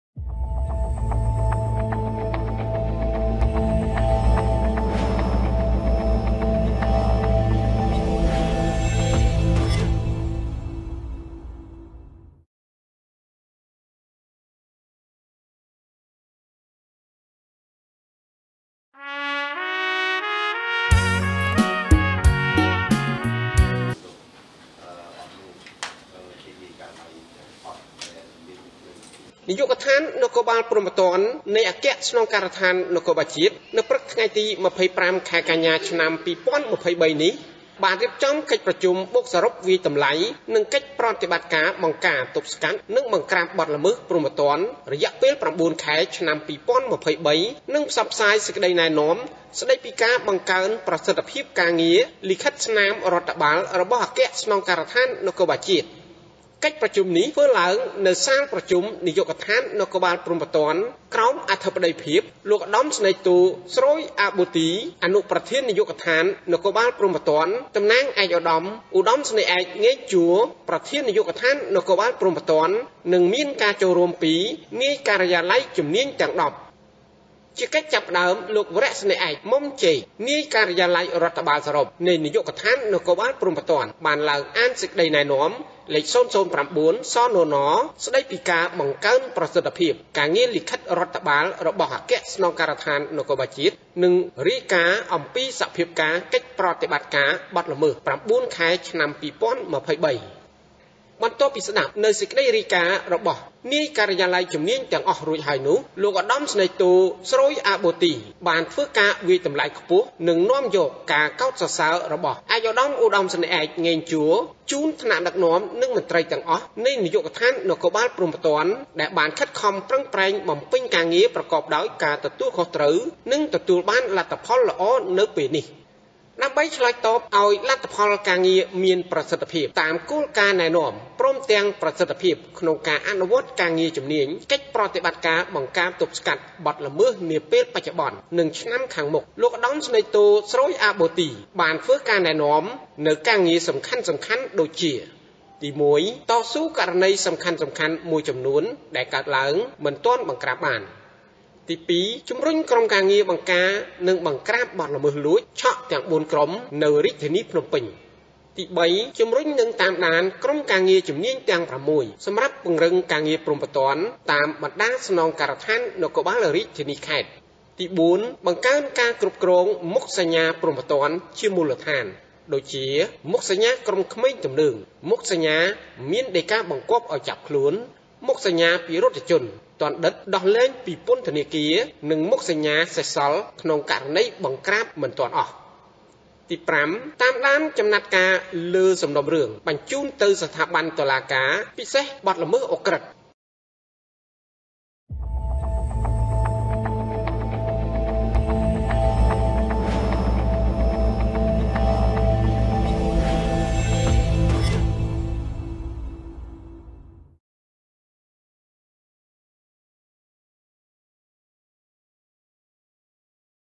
ប្រជុំបូកសរុបវាយតម្លៃប្រតិបត្តិការបង្ការ ទប់ស្កាត់ និងបង្ក្រាបបទល្មើសព្រហ្មទណ្ឌ រយៈពេល៩ខែ
នាយកដ្ឋាននគរបាលព្រហ្មទណ្ឌ នៃអគ្គស្នងការដ្ឋាននគរបាលជាតិ នៅព្រឹកថ្ងៃទី២៥ ខែកញ្ញា ឆ្នាំ២០២៣នេះ បានរៀបចំកិច្ចប្រជុំបូកសរុបវាយតម្លៃ កិច្ចប្រតិបត្តិការបង្ការ ទប់ស្កាត់ និងបង្ក្រាបបទល្មើសព្រហ្មទណ្ឌ រយៈពេល៩ខែ ឆ្នាំ២០២៣ និងផ្សព្វផ្សាយសេចក្ដីណែនាំ ស្ដីពីការបង្កើនប្រសិទ្ធភាពការងារលិខិតស្នាមរដ្ឋបាល របស់អគ្គស្នងការដ្ឋាននគរបាលជាតិ។
កិច្ចប្រជុំនេះ ធ្វើឡើងនៅសាលប្រជុំនាយកដ្ឋាននគរបាលព្រហ្មទណ្ឌ ក្រោមអធិបតីភាព លោកឧត្តមសេនីយ៍ទោ ស្រូយ អាប៊ូទី អនុប្រធាននាយកដ្ឋាននគរបាលព្រហ្មទណ្ឌ តំណាងឯកឧត្តម ឧត្តមសេនីយ៍ឯក ង៉េង ជួ ប្រធាននាយកដ្ឋាននគរបាលព្រហ្មទណ្ឌ និងមានការចូលរួមពីនាយការិយាល័យជំនាញទាំង១០។
ជាកិច្ចចាប់ផ្ដើម លោកវរសេនីយ៍ឯក មុំ ចេក នាយការិយាល័យរដ្ឋបាល-សរុប នៃនាយកដ្ឋាននគរបាលព្រហ្មទណ្ឌ បានឡើងអានសេចក្ដីណែនាំលេខ០០៩ សណន ស្ដីពីការបង្កើនប្រសិទ្ធភាពការងារលិខិតរដ្ឋបាល របស់អគ្គស្នងការដ្ឋាននគរបាលជាតិ និងរាយការណ៍អំពីសភាការណ៍កិច្ចប្រតិបត្តិការបទល្មើស ០៩ខែ ឆ្នាំ២០២៣។
បន្ទាប់ពីស្ដាប់នូវសេក្ដីរាយការណ៍របស់នាយការិយាល័យជំនាញទាំងអស់រួចរាល់ហើយនោះ, លោកឧត្តមសេនីយ៍ទោ ស្រូយ អាប៊ូទី បានធ្វើការវាយតម្លៃខ្ពស់ និងនាំយកការកោតសរសើររបស់ ឯកឧត្តម ឧត្តមសេនីយ៍ឯក ង៉េង ជួ ជូនថ្នាក់ដឹកនាំ និងមន្ដ្រីទាំងអស់នៃនាយកដ្ឋាននគរបាលព្រហ្មទណ្ឌ ដែលបានខិតខំប្រឹងប្រែងបំពេញការងារប្រកបដោយការទទួលខុសត្រូវ និងទទួលបានលទ្ធផលល្អនៅពេលនេះ។
ដើម្បីឆ្លើយតបឲ្យលទ្ធផលការងារមានប្រសិទ្ធភាពតាមគោលការណ៍ណែនាំ ព្រមទាំងប្រសិទ្ធភាពក្នុងការអនុវត្តការងារជំនាញកិច្ចប្រតិបត្តិការបង្ការ ទប់ស្កាត់បទល្មើសនាពេលបច្ចុប្បន្ន និងឆ្នាំខាងមុខ លោកឧត្តមសេនីយ៍ទោ ស្រូយ អាប៊ូទី បានធ្វើការណែនាំនូវការងារសំខាន់ៗ ចំនួនដូចជា៖
*ទី១៖ តស៊ូករណីសំខាន់ៗមួយចំនួន ដែលកើតឡើងមិនទាន់បង្ក្រាបបាន
*ទី២៖ ជំរុញក្រុមការងារបង្ការ និងបង្ក្រាបបទល្មើសលួច ឆក់ទាំង៤ក្រុម នៅរាជធានីភ្នំពេញ
*ទី៣៖ ជំរុញ និងតាមដានក្រុមការងារជំនាញទាំង៦ សម្រាប់ពង្រឹងការងារព្រហ្មទណ្ឌតាមបណ្ដាស្នងការដ្ឋាននគរបាលរាជធានី-ខេត្ត
*ទី៤៖ បង្កើnការងារគ្រប់គ្រងមុខសញ្ញាព្រហ្មទណ្ឌជាមូលដ្ឋាន ដូចជា៖ មុខសញ្ញាក្រុមក្មេងទំនើង, មុខសញ្ញាមានដីកាបង្គាប់ឲ្យចាប់ខ្លួន, មុខសញ្ញាពិរុទ្ធជន ទណ្ឌិតដោះលែងពីពន្ធនាការ និងមុខសញ្ញាសេសសល់ក្នុងករណីបង្ក្រាមមិនទាន់អស់
*ទី៥៖ តាមដានចំណាត់ការលើសំណុំរឿងបានបញ្ជូនទៅស្ថាប័នតុលាការ ពិសេសបទល្មើសឧក្រិដ្ឋ៕